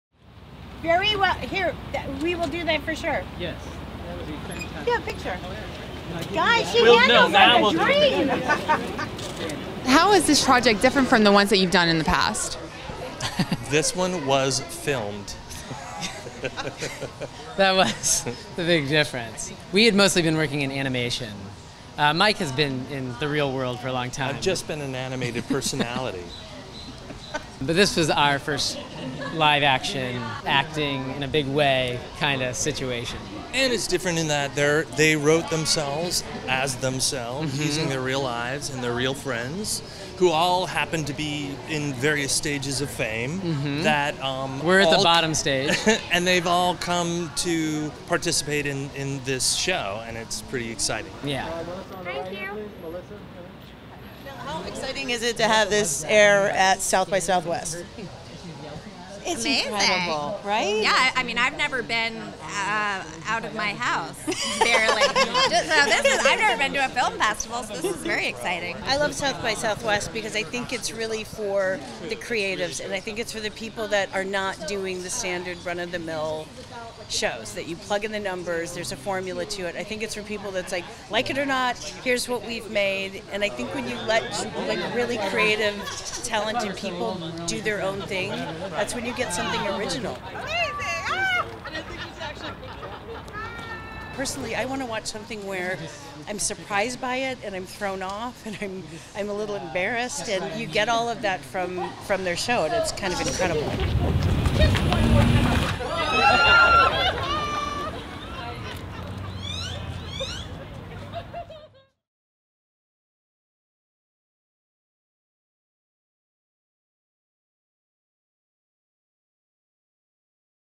Melissa McCarthy takes "Nobodies" cast for a spin at SXSW 2017
“Nobodies” executive producer Melissa McCarthy took the TV Land show’s cast for a spin in an electric car at the comedy’s premiere screening at red carpet at the South by Southwest Film Festival in Austin, Tx, on Monday, March 13, 2017.